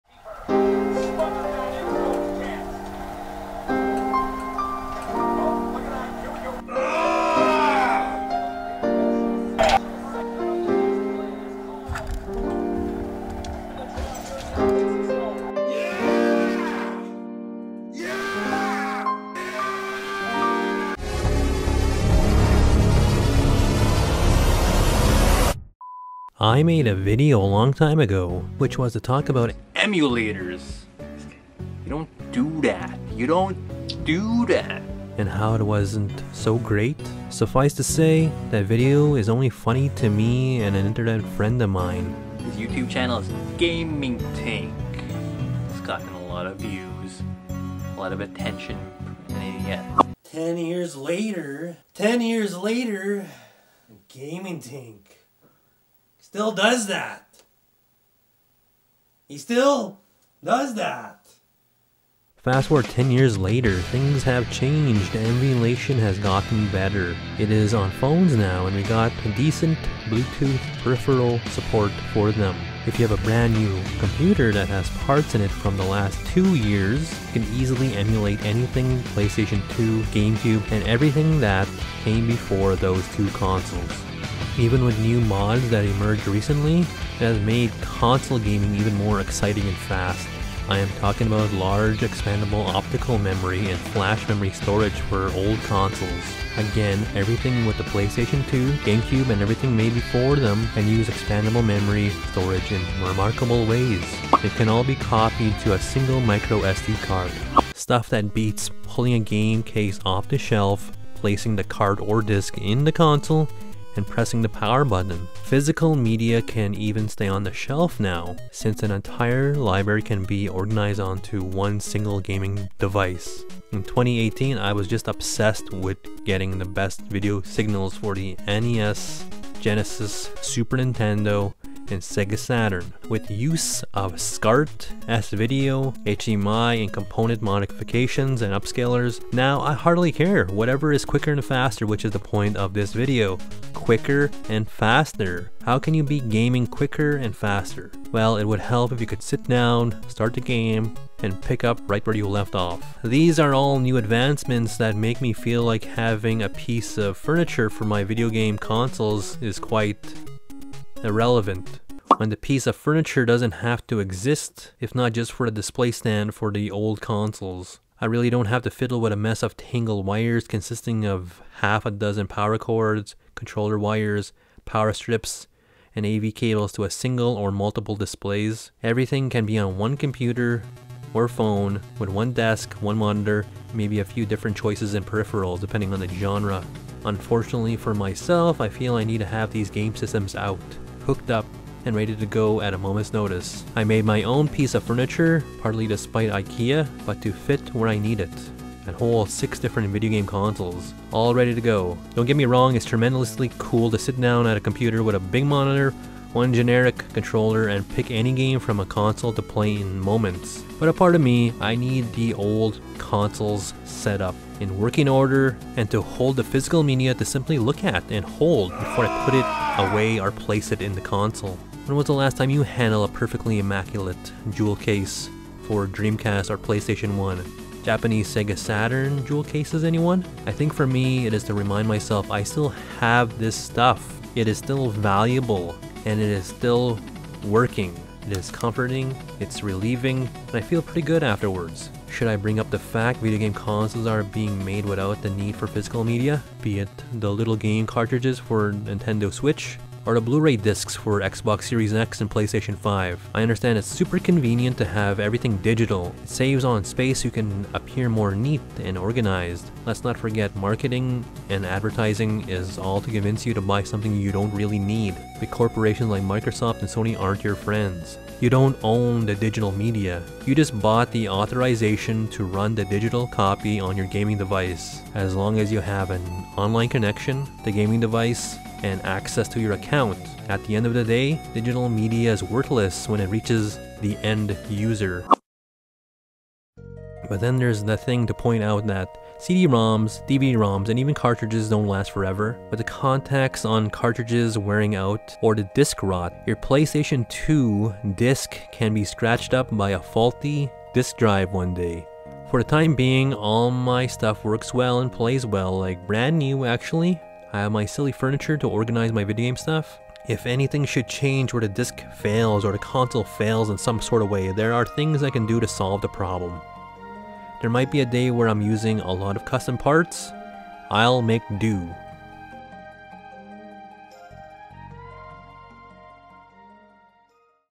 Then & Now: Importance of Physical Media
Dark Morris, the wolf behind the odyssey is here with a new video. This time, its about emulation & digital media vs physical media. Why have multiple consoles when you can have one gaming device? Why have hundreds of game cases when it can all be stored on a micro sd card? Why have do consoles even need a disc tray anymore? Computer cases aren't even made with a internal disc drive anymore.